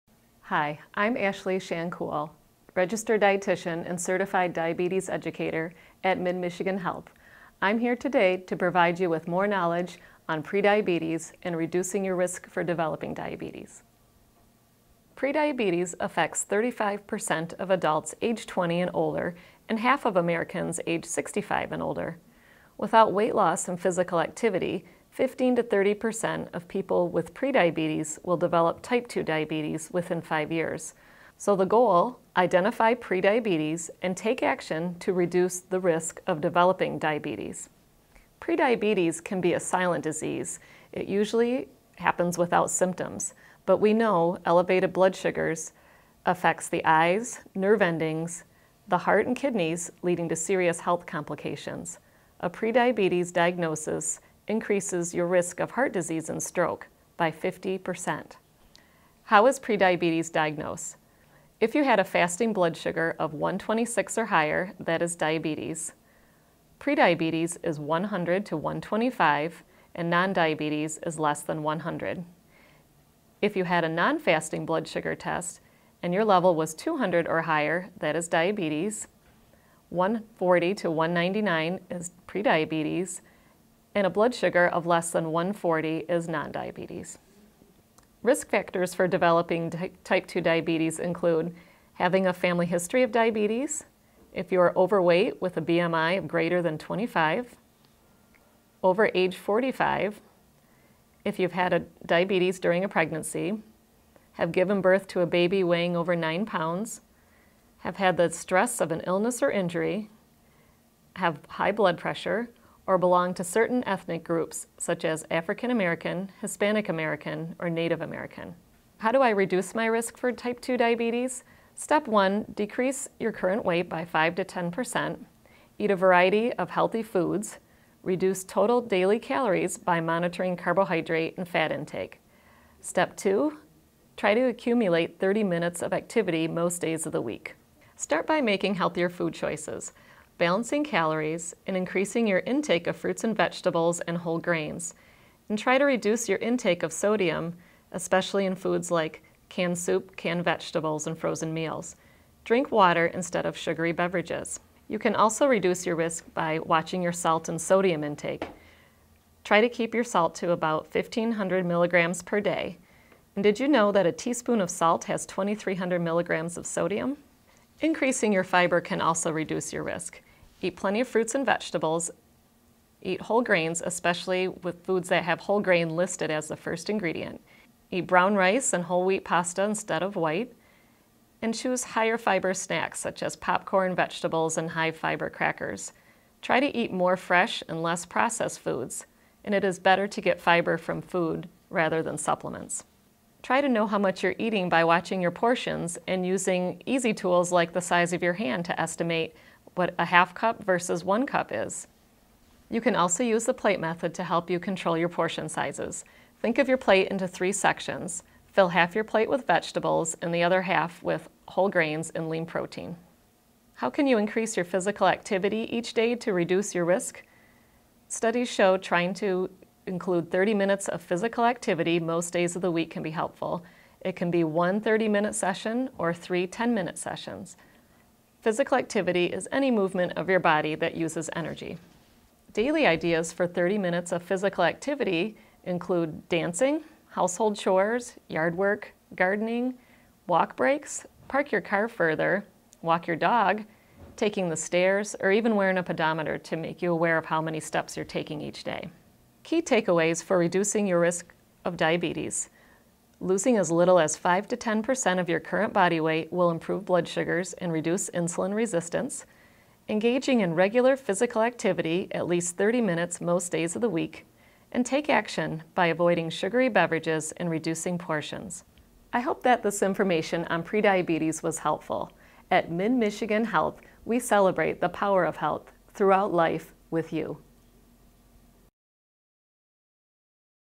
Diabetes Prevention YouTube
More than 86 million Americans have prediabetes and are at risk of developing diabetes. Only about 9 million people know they have prediabetes, a potentially reversible condition. Prediabetes occurs when blood sugar levels are higher than normal but not high enough for a type 2 diabetes diagnosis. Risk for developing type 2 diabetes may be reduced or eliminated by weight loss, healthier eating, and increased physical activity. Ashley Shankool R.D., C.D.E., registered dietitian and certified diabetes educator at MidMichigan Health discusses modifiable risk factors for diabetes and provides practical tips on how to reduce your overall risk of developing this disease.